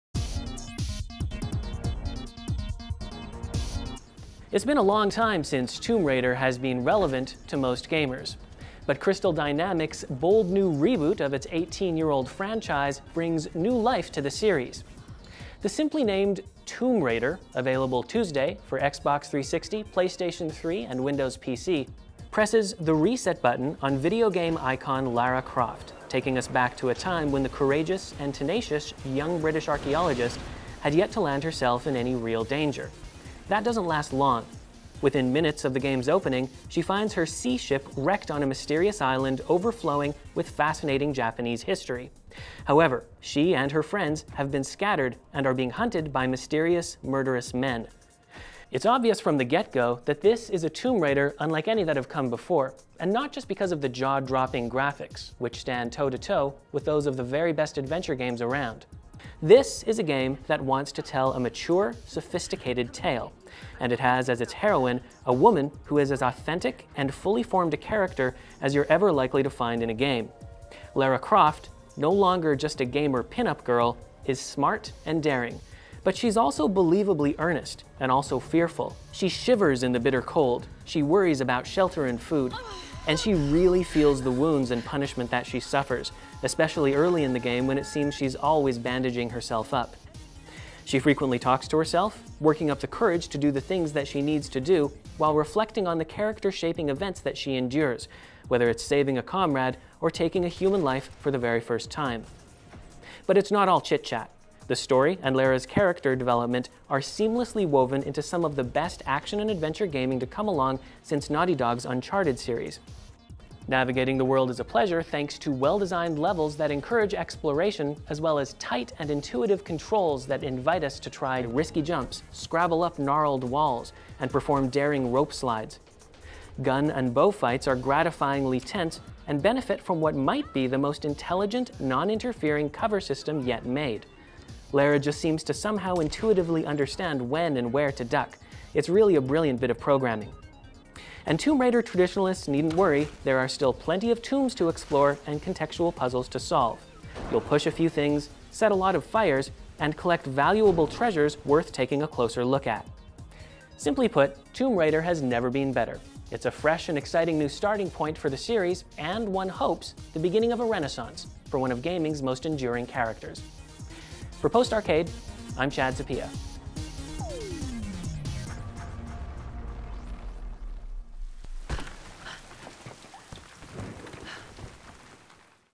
Post Arcade: Tomb Raider Review
Post Arcade Columnist Chad Sapieha takes you inside the world of the new Tomb Raider reboot, the latest title from Square Enix and Crystal Dynamics. Tomb Raider launches in Canada on March 5, 2013 and is available for PC, Xbox 360 and PlayStation 3.